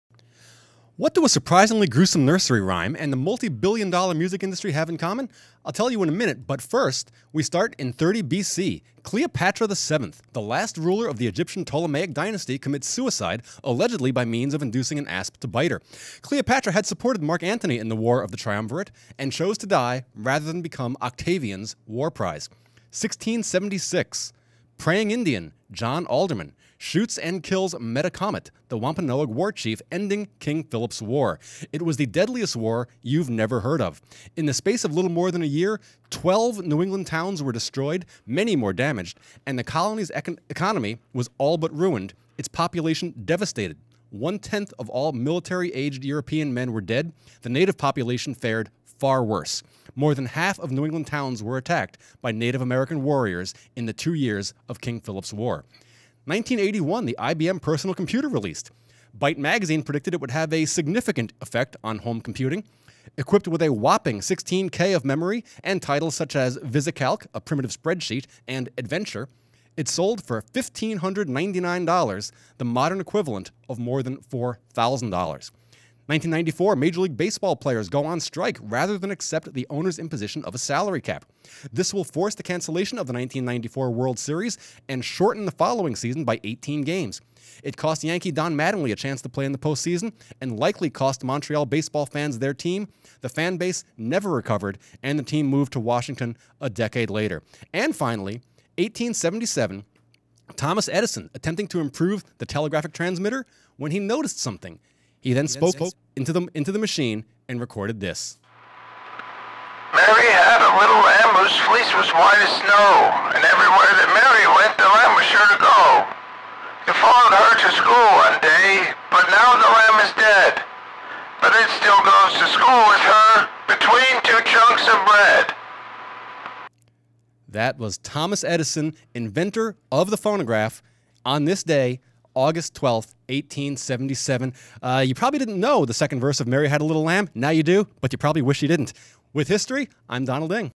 Today in History: August 12
Donald Eng of the HAN Network takes a look back at the big events of August 12th. Including what a surprisingly gruesome nursery rhyme and the multi-billion dollar music industry have in common...